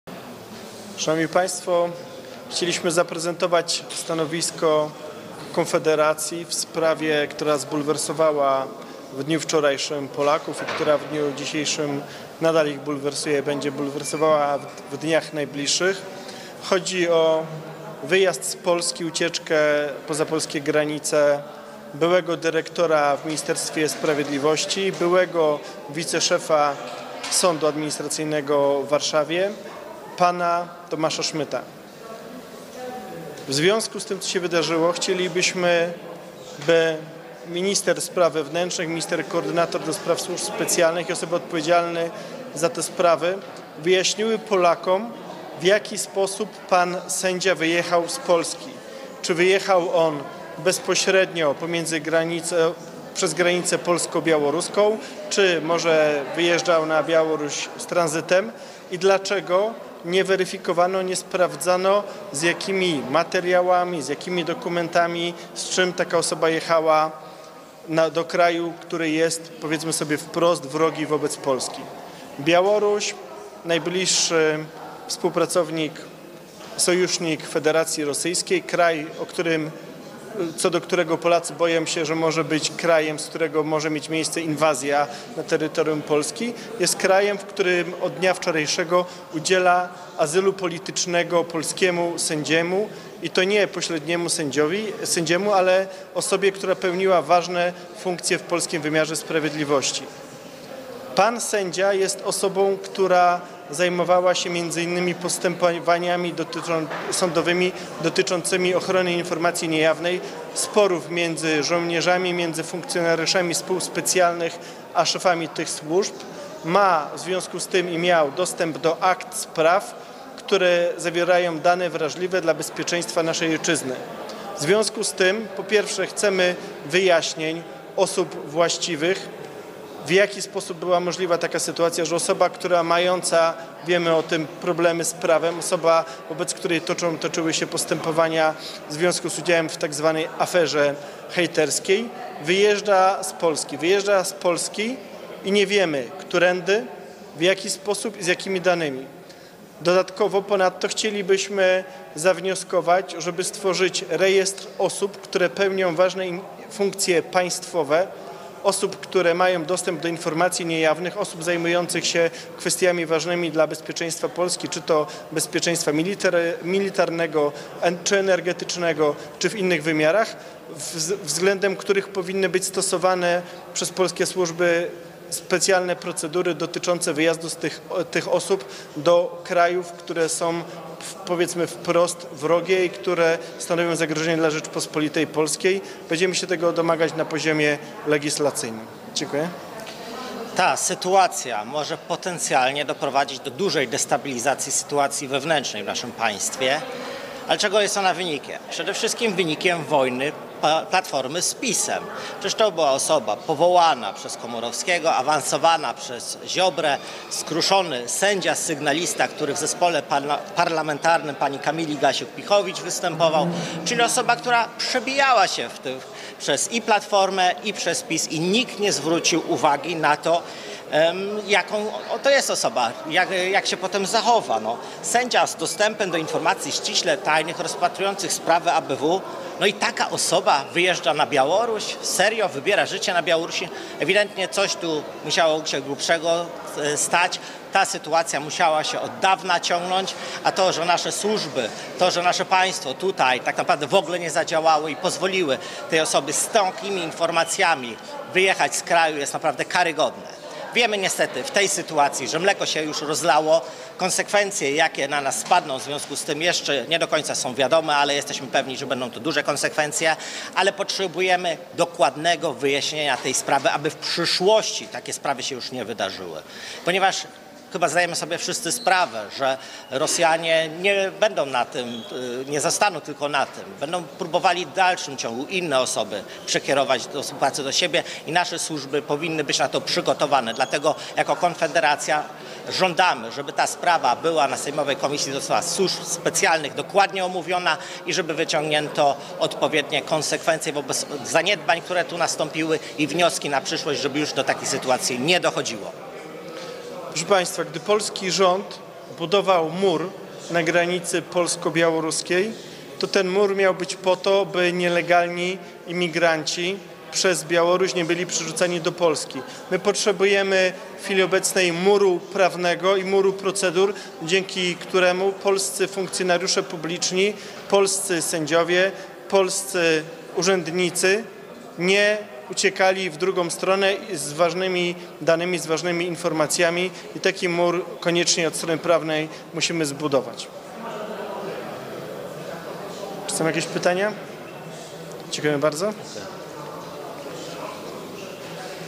Konfederacja: Współpracownik Ziobry i Gasiuk-Pihowicz rosyjskim agentem?
Przemysław Wipler
Krystian Kamiński